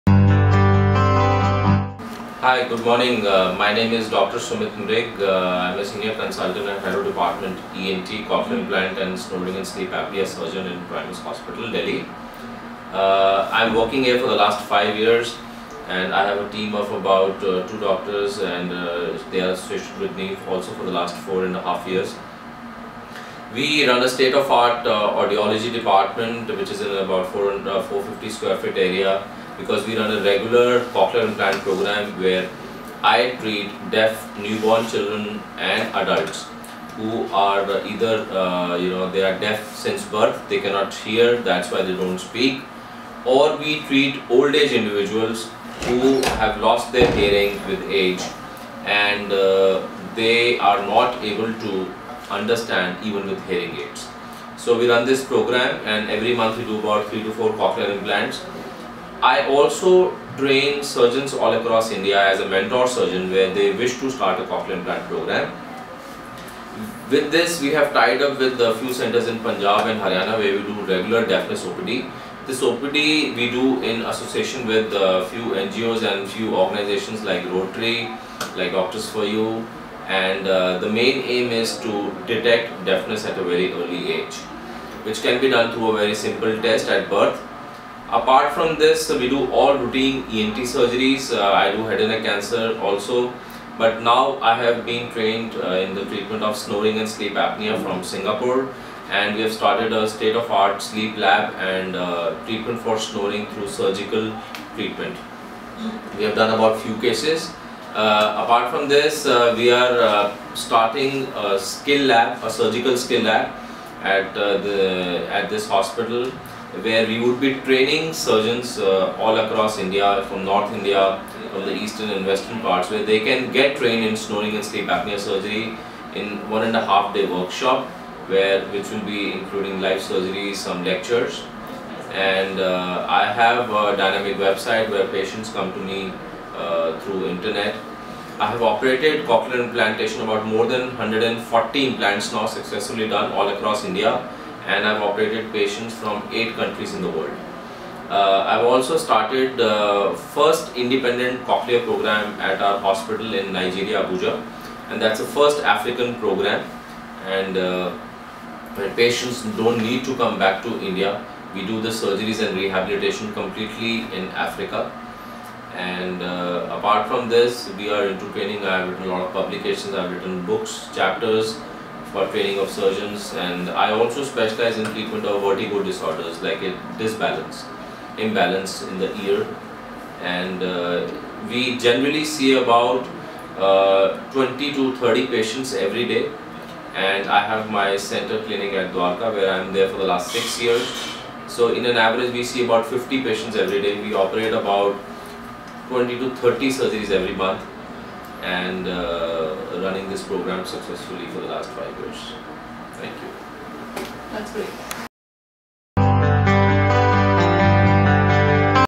Dr. Sumit Mrig - Treatment of Deafness in Children, Adults & Elderly
Dr. Sumit Mrig, Senior Consultant - ENT & Cochlear Implant Surgeon, Primus Hospital talks in detail about Audiology department and cochlear implant program in Primus Hospital for treatment of deafness in young children, adults and elder people, and his experience in cochlear implants, with more than 140 successful cochlear implants, in patients from 8 countries. He also mentions about deafness OPD in various locations in Punjab for screening & early detection of deafness, and cochlear implant program in Africa.